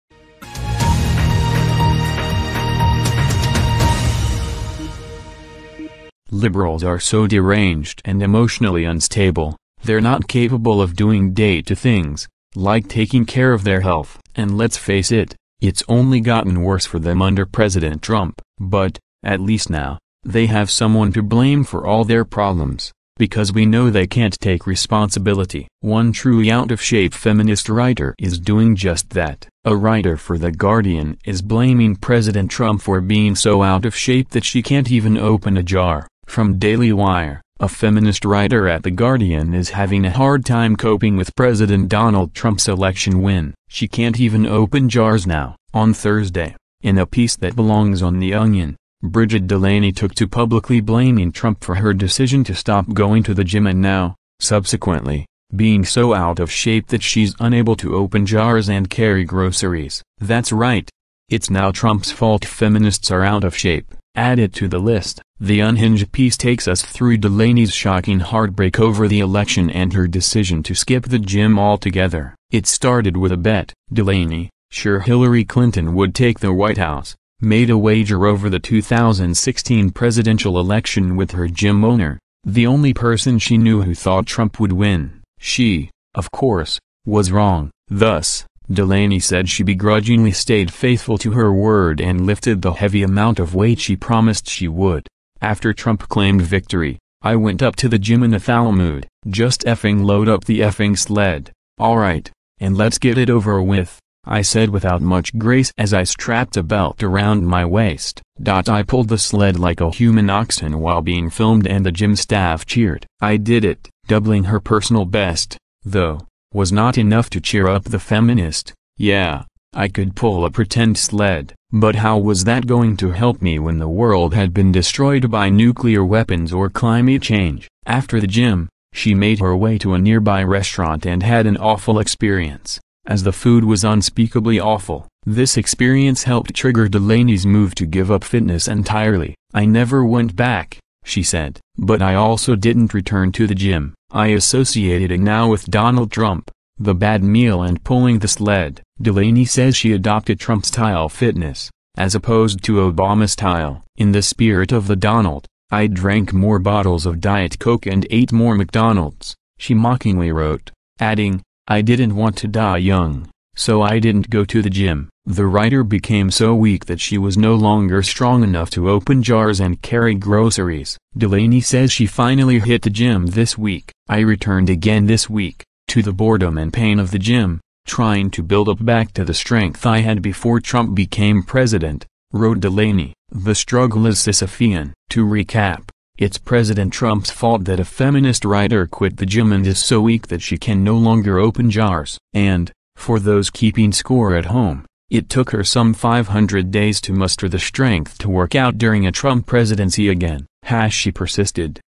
WHAT THE? Feminist Writer Blames Trump for Being GROSSLY Out of Shape
Liberals are so deranged and emotionally unstable, they’re not capable of doing day to things, like taking care of their health.

And let’s face it, it’s only gotten worse for them under President Trump.

But, at least now, they have someone to blame for all their problems, because we know they can’t take responsibility.

One truly out of shape feminist writer is doing just that.

A writer for the “Guardian” is blaming President Trump for being so out of shape that she can’t even open a jar.

From Daily Wire

A feminist writer at The Guardian is having a hard time coping with President Donald Trump’s election win. She can’t even open jars now.

On Thursday, in a piece that belongs on The Onion, Brigid Delaney took to publicly blaming Trump for her decision to stop going to the gym and now, subsequently, being so out of shape that she’s unable to open jars and carry groceries.

That’s right, it’s now Trump’s fault feminists are out of shape. (Add it to the list!)

The unhinged piece takes us through Delaney’s shocking heartbreak over the election and her decision to skip the gym altogether. It started with a bet. Delaney, sure Hillary Clinton would take the White House, made a wager over the 2016 presidential election with her gym owner, the “only person” she knew who thought Trump would win. She, of course, was wrong. Thus, Delaney said she begrudgingly stayed faithful to her word and lifted the heavy amount of weight she promised she would:

After Trump claimed victory, I went up to the gym in a foul mood. “Just f***ing load up the f***ing sled, all right, and let’s get it over with,” I said without much grace as I strapped a belt around my waist. … I pulled the sled like a human oxen while being filmed and the gym staff cheered. I did it.

Doubling her personal best, though, was not enough to cheer up the feminist: “Yeah, I could pull a pretend sled. But how was that going to help me when the world had been destroyed by nuclear weapons or climate change?”

After the gym, she made her way to a nearby restaurant and had an awful experience, as the food was “unspeakably awful.” This experience helped trigger Delaney’s move to give up fitness entirely.

“I never went back,” she said. “But I also didn’t return to the gym. I associated it now with Donald Trump, the bad meal and pulling the sled.”

Delaney says she adopted Trump-style fitness, as opposed to Obama-style. “In the spirit of the Donald, I drank more bottles of Diet Coke and ate more McDonald’s,” she mockingly wrote, adding, “I didn’t want to die young, so I didn’t go to the gym.”

The writer became so weak that she was no longer “strong enough to open jars and carry groceries.”

Delaney says she finally hit the gym this week.

“I returned again this week – to the boredom and pain of the gym – trying to build up back to the strength I had before Trump became president,” wrote Delaney. “The struggle is Sisyphean.”

To recap: It’s President Trump’s fault that a feminist writer quit the gym and is so weak that she can no longer open jars. And, for those keeping score at home, it took her some 500 days to muster the strength to work out during a Trump presidency again.